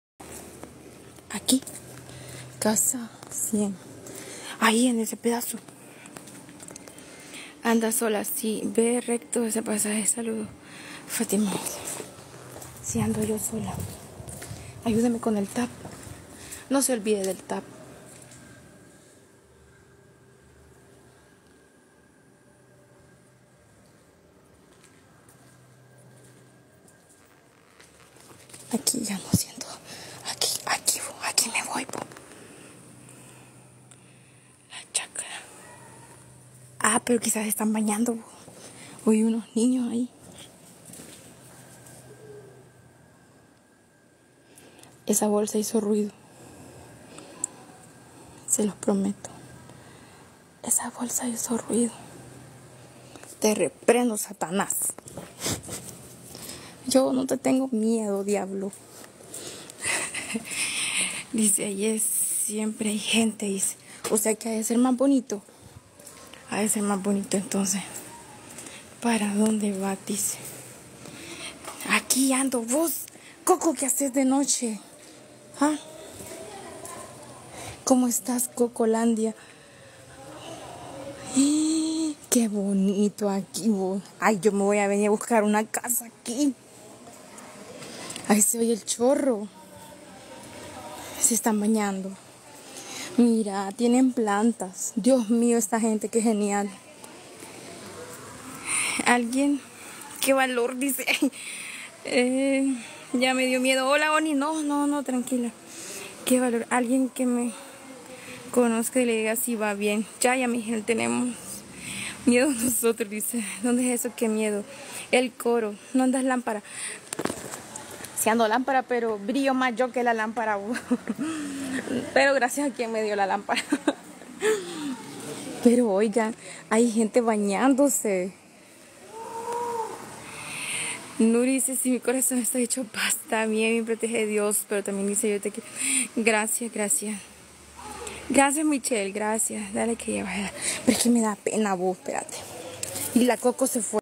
SAN SALVADOR DE NOCHE COMUNIDAD. EL CORO, comunidad con poza de aguas cristalinas 🌊🇸🇻
Después de un año volvemos con las caminatas nocturnas conociendo San Salvador de noche y más colonias o comunidades que antes eran peligrosas ahora nos alegra que los Salvadoreños duerman con más tranquilidad
también tratamos de hacer turismo conociendo lugares tan bellos que tiene nuestro El Salvador
gracias por unirte a este canal
buscamos en Tiktok como @aventuraleza_sv
transmitimos de noche bienvenidos 💙🇸🇻 a este nuevo El Salvador 💙